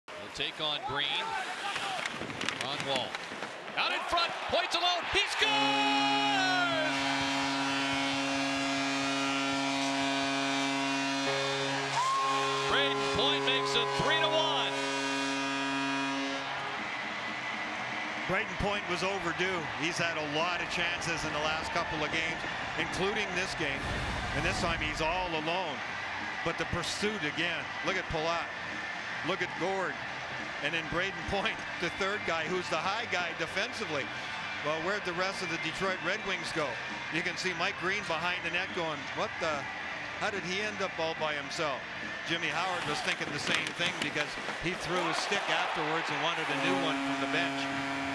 Braydon Point Goal vs DET October 27, 2017
Braydon Point 5th goal of 2017-18 nhl season. Assisted by Yanni Gourde and Ondrej Palat